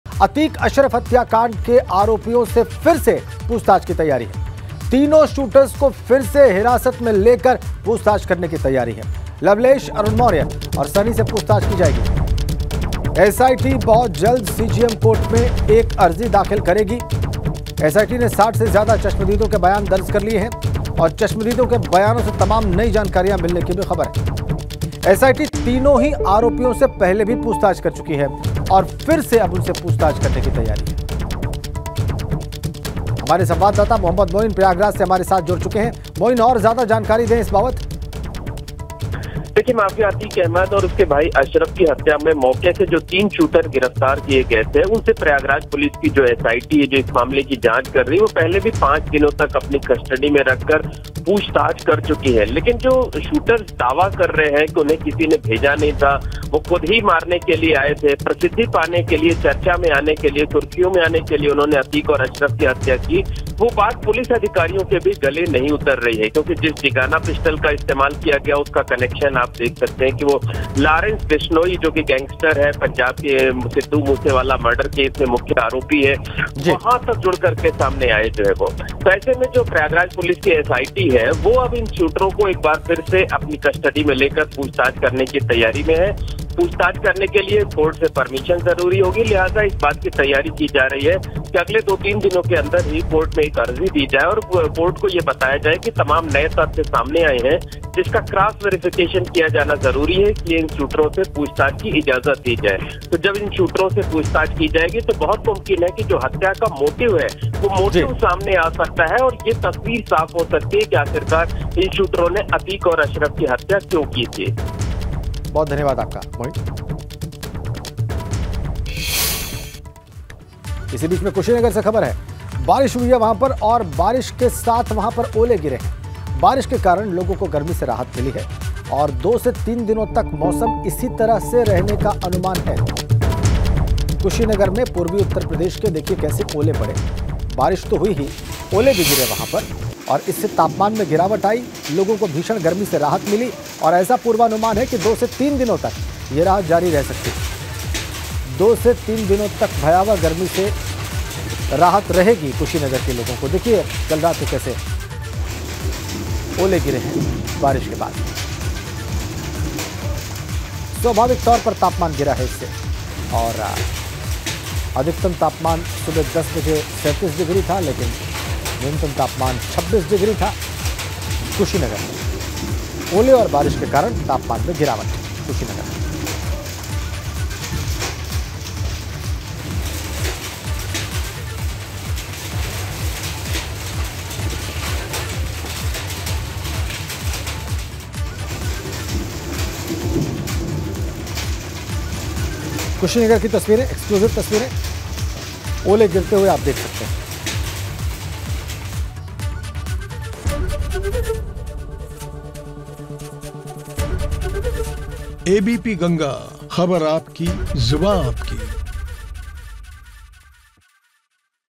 Atiuqe-Ashraf के आरोपियों से फिर से पुछताछ की तैयारी, SIT जल्द कोर्ट में दाखिल करेगी अर्जी
अतीक-अशरफ के तीनों आरोपियों पर फिर से पुछताछ करने की तैयारी हो रही है. बता दें कि एसआईटी जल्द ही सीजेएम कोर्ट में अर्जी दाखिल करेगी और तीनों आरोपियों को हिरासत में लेकर पुछताछ कर सकती है. देखिए पूरी रिपोर्ट